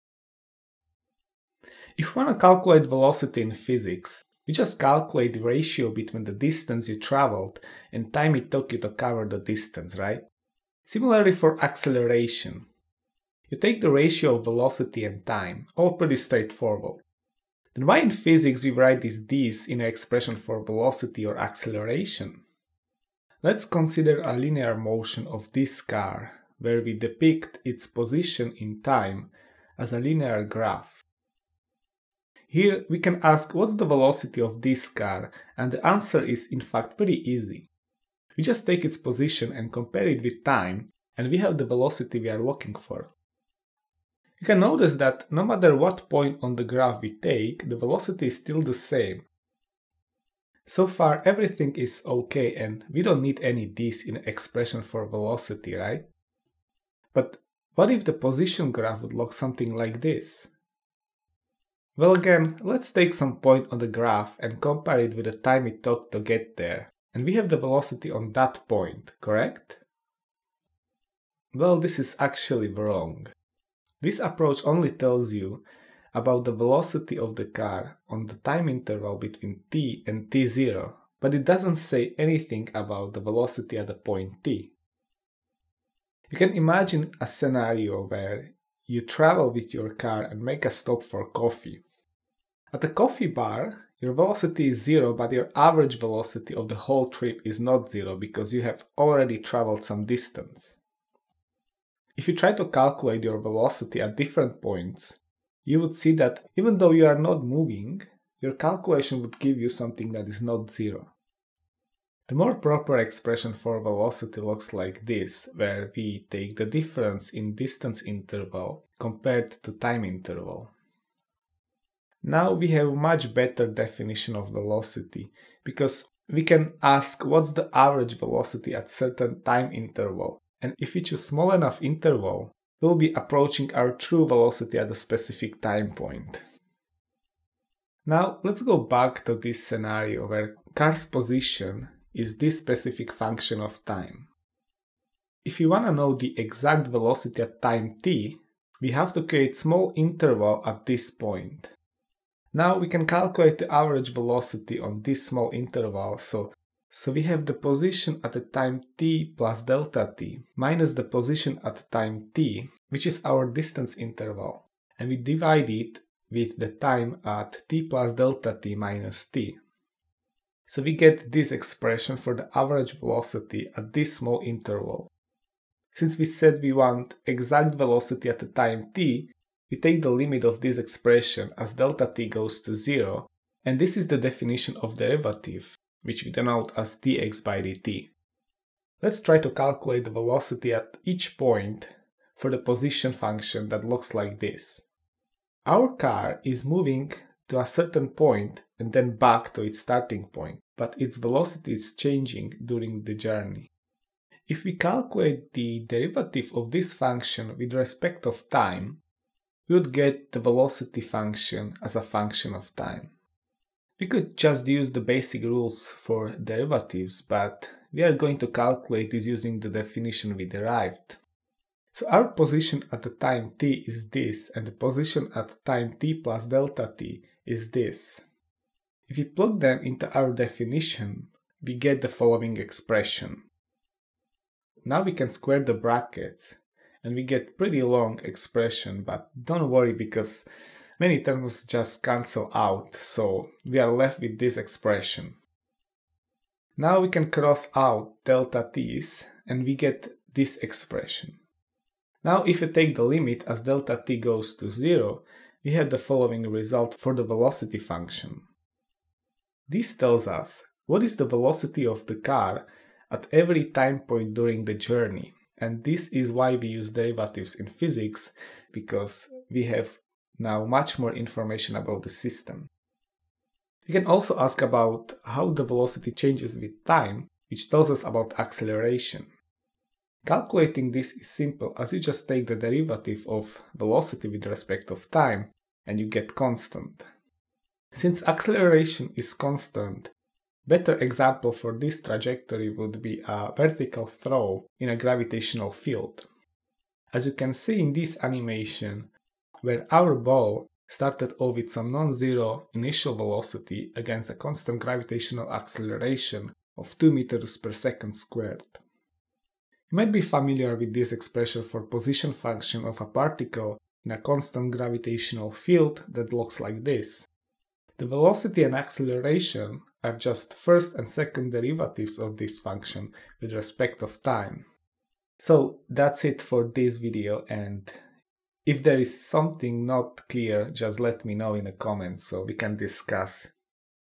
Why we need DERIVATIVES in Physics??!!
You might wonder why we write those d's in the expression for velocity or acceleration or many other things in physics. Especially if you just started more advanced courses in mechanics. This video gives intuitive explanation and also some simple example on this topic.